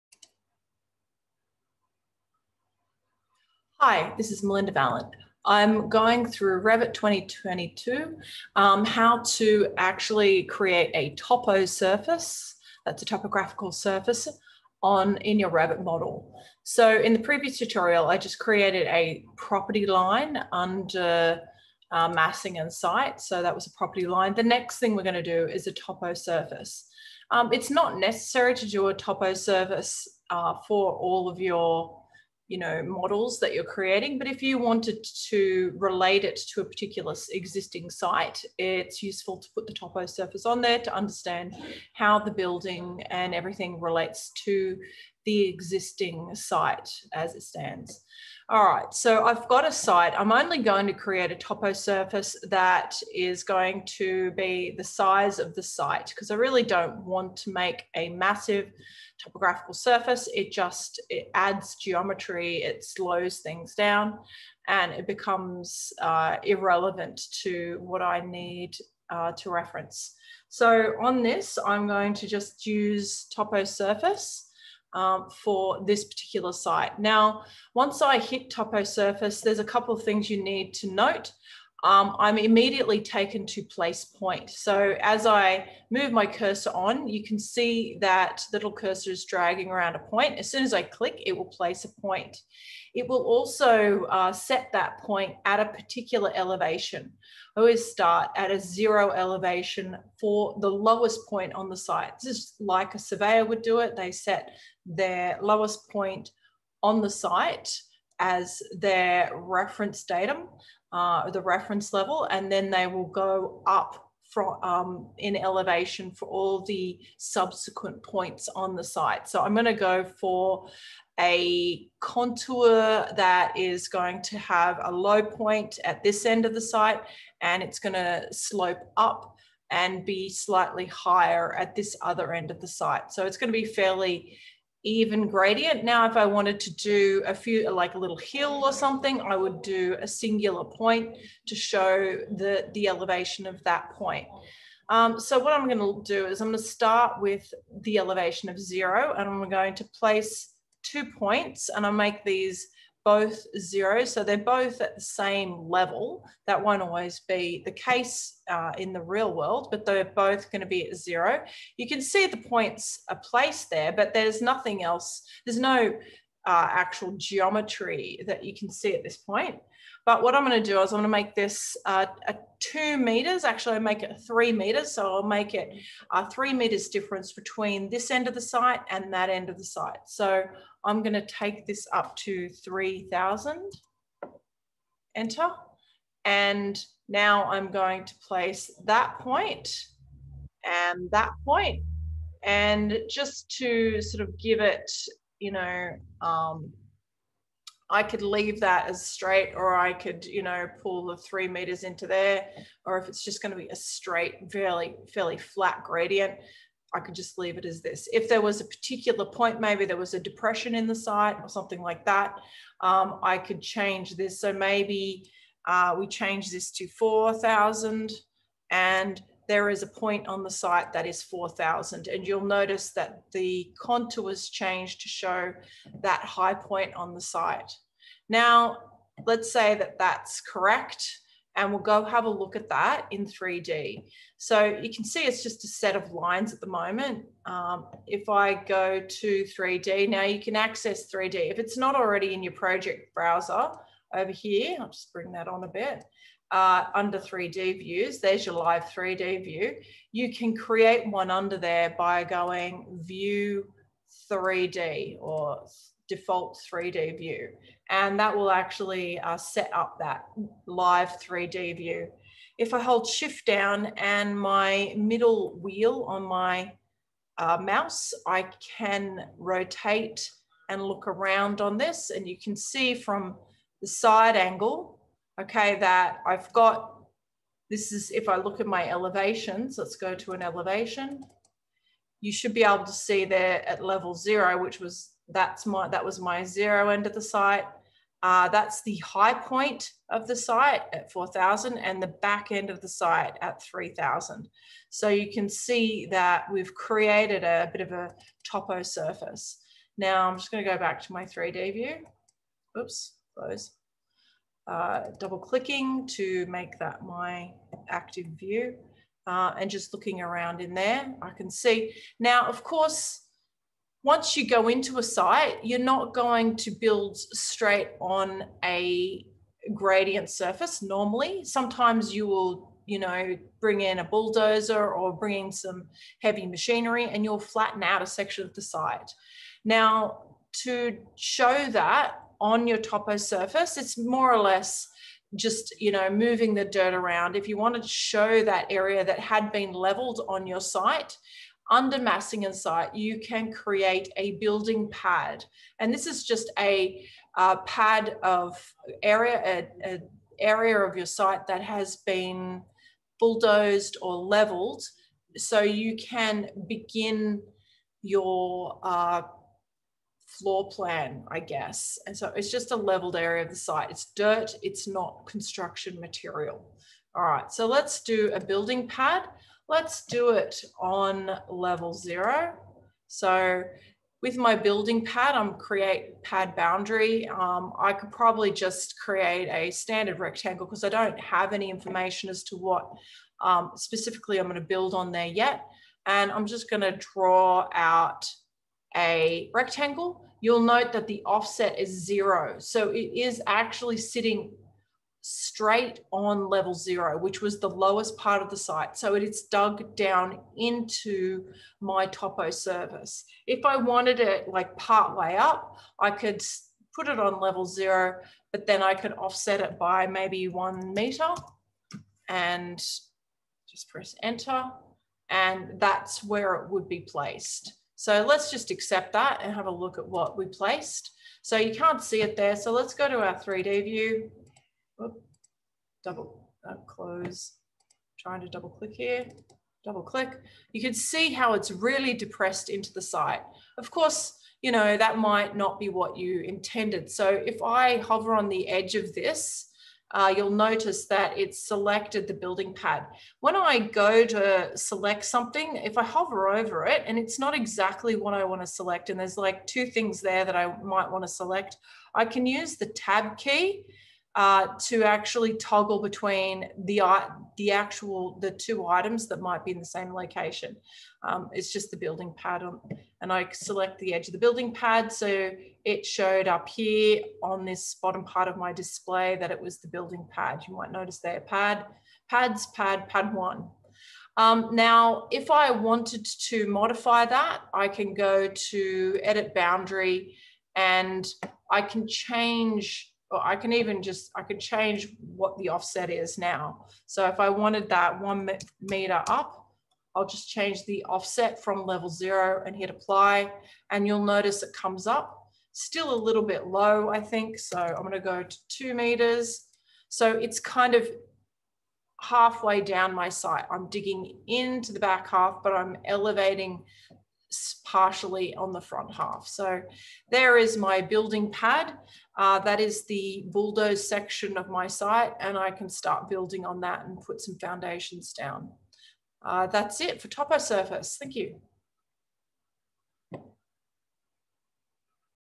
Revit 2022 #3 Topographical Surface and Building Pad
I use the Massing & Site tool Toposurface to create a simple topographical contoured site and add a building pad leveled area.